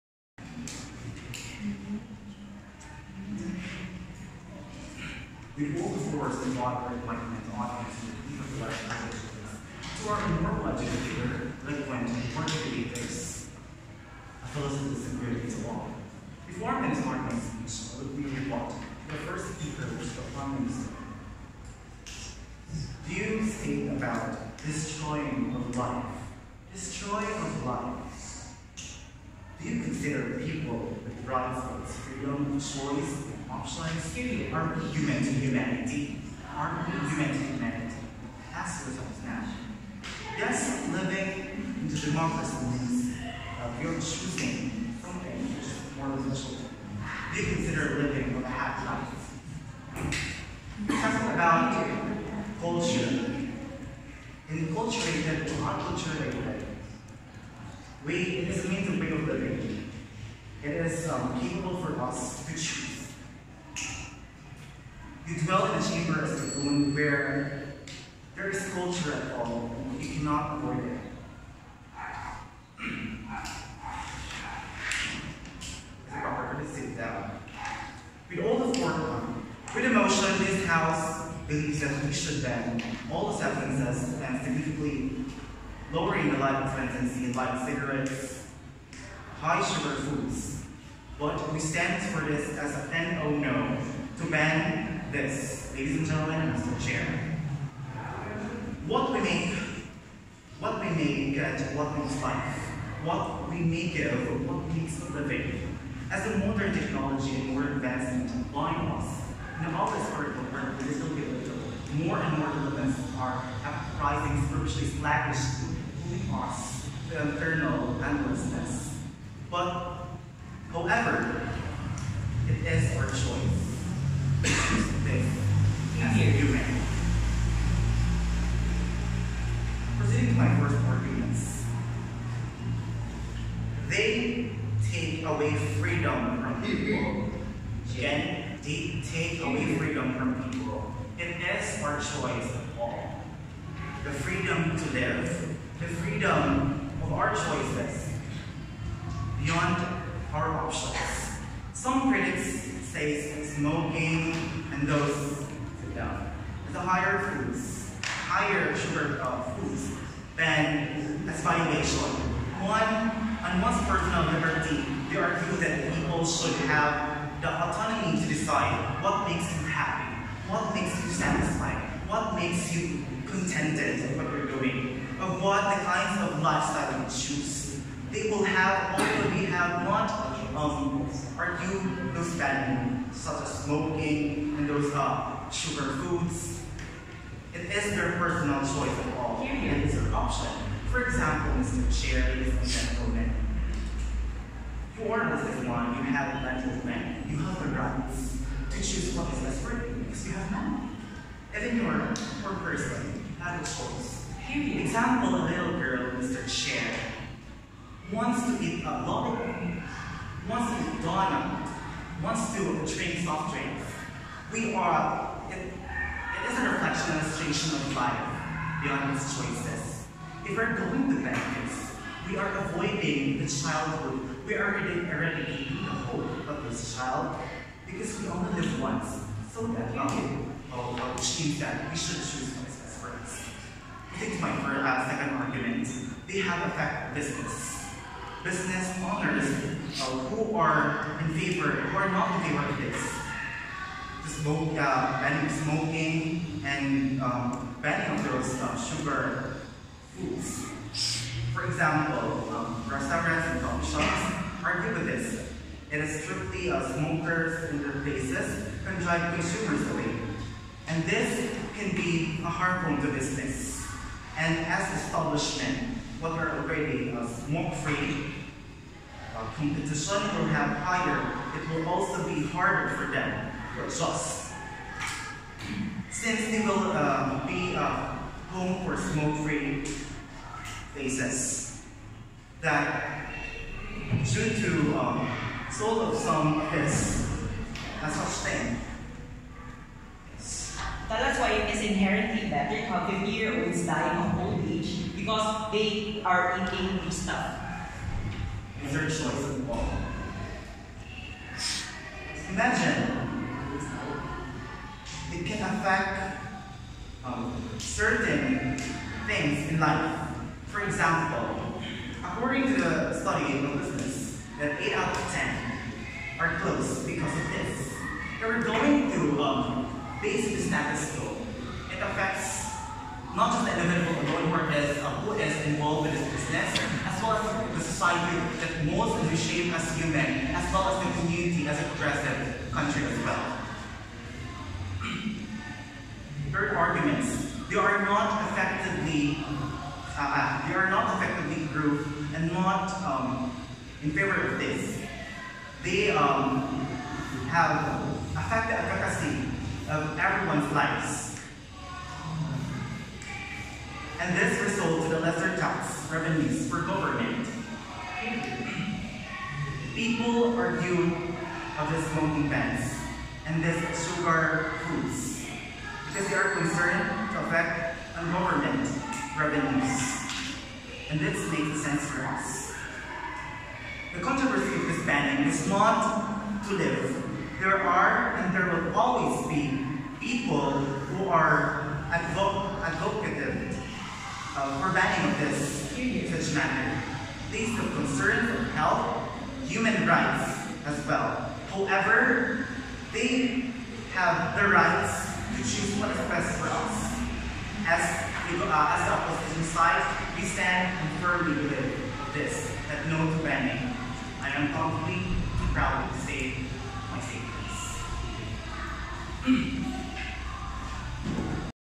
Round 1 - LO - 5th BIPDC - (Room 4)
Motion: THBT we should ban all substances that significantly lower life expectancy (e.g. cigarettes, high sugar foods, etc)

Affirmative: Bukidnon State University A
Negative: Valencia National High School A

Winner: Affirmative

The 5th Bukidnon Interhighschool Parliamentary Debate Championship is hosted by Bukidnon State University Debate Varsity (BukSU DV).